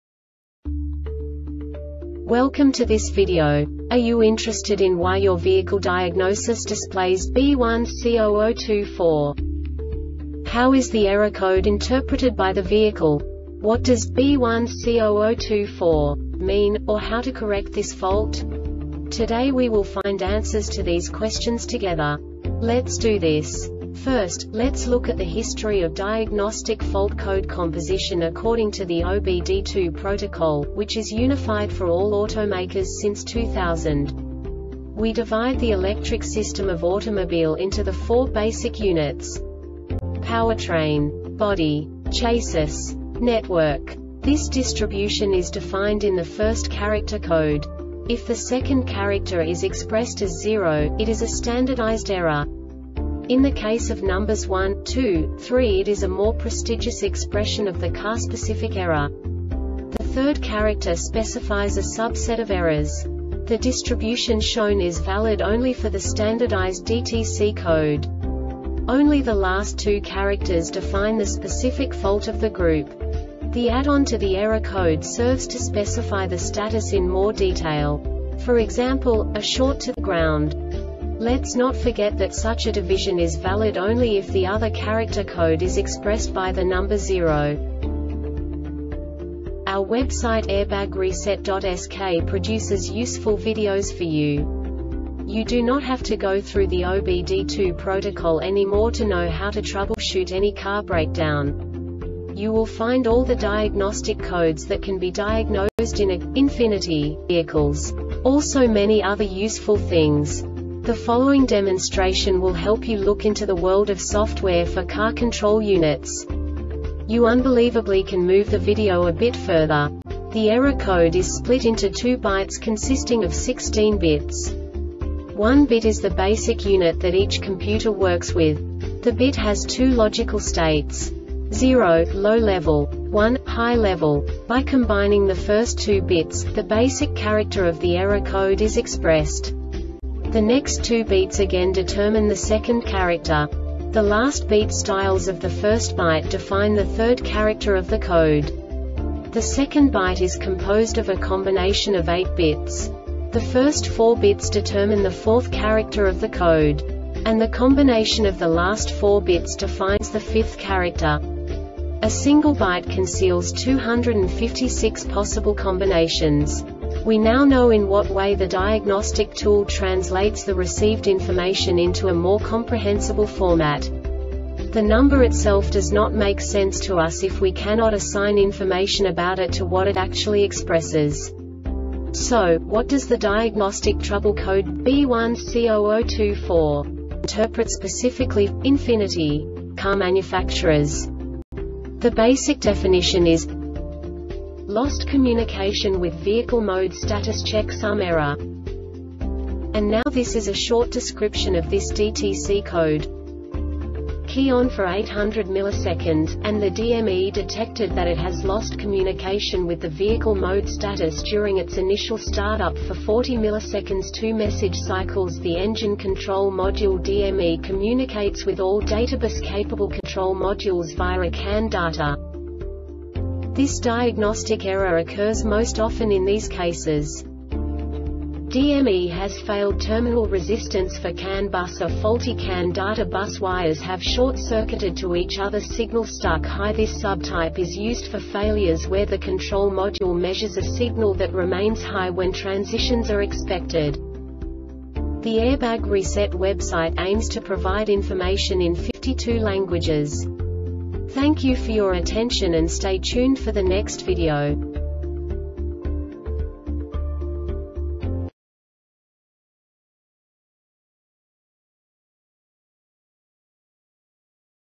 DTC Ford B1C00-24 Short Explanation
Contents:
0:21 Basic DTC analysis according to OBD2 protocol standard.
1:48 Insight into programming
2:58 A diagnostically understandable description of the Diagnostic Trouble Code
3:05 Basic error definition

Make:
Ford
Code:
B1C00 24
Definition: